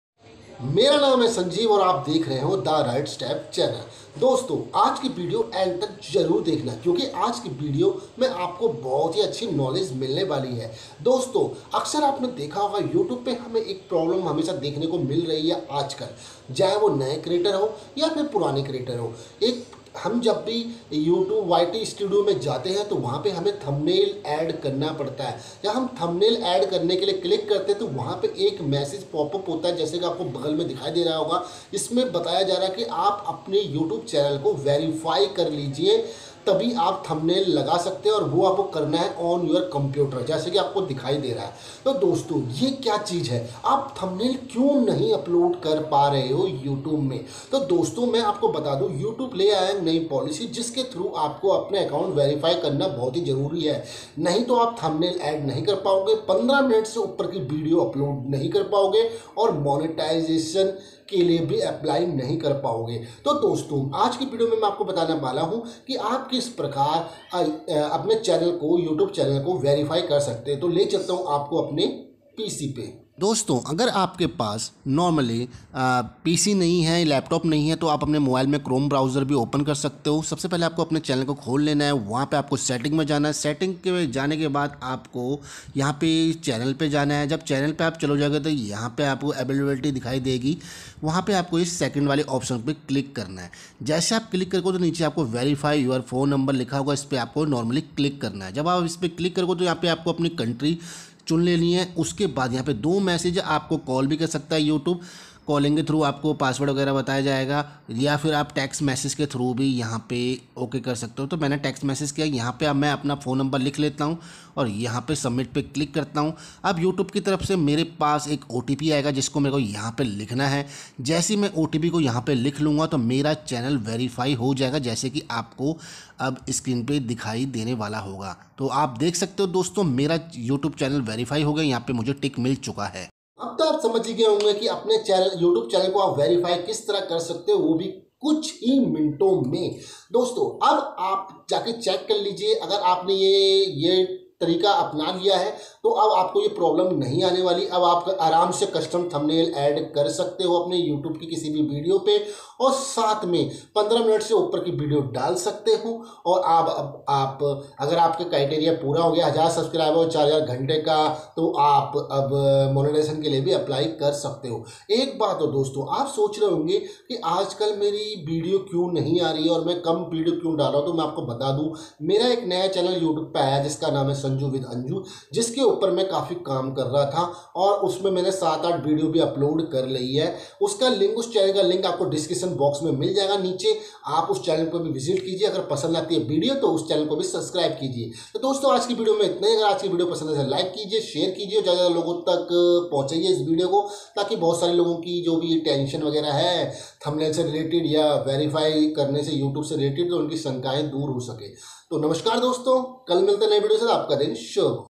How to Verify Your Youtube Account / Verify Your Youtube Account 2021 ?
aaj ki video me hum aap ko bata rahai hai ki aap apnai youtube channel ko verify kaise kar saktai ho. jaada jaankri k lea video ko end tak jarur dekhai aur jaada se jaada logo ko share kare.. video pasend aayai to plz like,comment and share jarur kare , channel ko subscribe karna na bhulai.